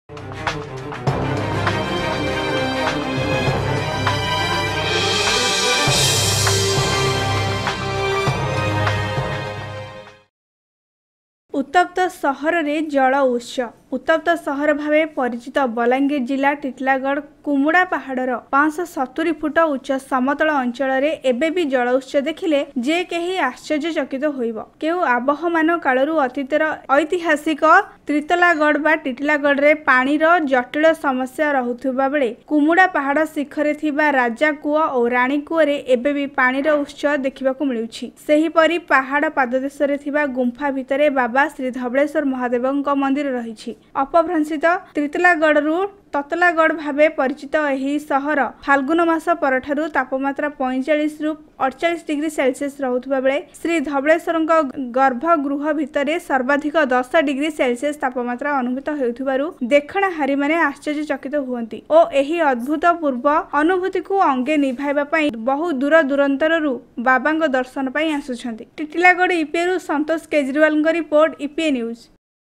Surprisingly the hot springs of the hot city , ଆଶ୍ଚର୍ଯ୍ୟ କରୁଛି ଉତ୍ତପ୍ତ ସହରର ଜଳଉତ୍ସ
ଉତ୍ତପ୍ତ ସହର ଭାବେ ପରିଚିତ ବଲାଙ୍ଗୀର ଜ଼ିଲ୍ଲା ର ଟିଟିଲାଗଡ଼  କୁମୁଡ଼ା ପାହାଡ ର 570 ଫୁଟ ଉଚ୍ଚ ସମତଳ ଅଞ୍ଚଳ ରେ ଏବେ ବି ଜଳଉତ୍ସ ଦେଖିଲେ ଯେ କେହି ବି ଆଶ୍ଚର୍ଯ୍ୟ ଚକିତ ହେବ।କେଉଁ ଆବାହମାନ କାଳରୁ  ଅତୀତର ଏହି ଐତିହାସିକ ତ୍ରିତଲାଗଡ(ଟିଟିଲାଗଡ)ରେ ପାଣିର ଜଟିଳ ସମସ୍ଯା ରହୁ ଥିବାବଳେ କୁମୁଡା ପାହାଡ ଶିଖର ରେ ଥିବା ରାଜା କୂଅ ଓ ରାଣୀକୂଅ ରେ ଏବେ ବି ପାଣି ର ଉତ୍ସ ଦେଖିବା କୁ ମିଳୁଛି। ସେହି ପରି ପାହାଡ ପାଦଦେଶ ରେ ଥିବା ଗୁମ୍ଫା ଭିତରେ ବାବା ଶ୍ରୀଧବଳେଶ୍ବର ମହାଦେବ ଙ୍କ ମନ୍ଦିର ରହିଛି।ଅପଭ୍ରଂସିତ ତ୍ରିତଲାଗଡ ରୁ ତତଲାଗଡ ଭାବେ ପରିଚିତ ଏହି ସହର ରେ ଫାଲ୍ଗୁନ ମାସ ପରଠାରୁ ତାପମାତ୍ରା45 ରୁ 48 ଡିଗ୍ରୀ ସେଲ୍ସିୟସ ରହୁଥିବା ବେଳେ ଶ୍ରୀଧବଳେଶ୍ବର ଙ୍କ ଗର୍ଭଗୃହ ଭିତରେ ସର୍ବାଧିକ 10 ଡିଗ୍ରୀ ସେଲ୍ସିୟସ ତାପମାତ୍ରା ଅନୁଭୂତ ହେଉଥିବା ରୁ ଦେଖଣା ହାରି ମାନେ ଆଶ୍ଚର୍ଯ୍ୟ ଚକିତ ହୁଅନ୍ତି ଓ ଏହି ଅଦ୍ଭୁତପୂର୍ବ ଅନୁଭୂତି କୁ ଅଙ୍ଗେ ନିଭାଇବା ପାଇଁ ବହୁ ଦୂର ଦୁରାନ୍ତର ରୁ ବାବା ଙ୍କ ଦର୍ଶନ ପାଇଁ ଆସୁଛନ୍ତି।।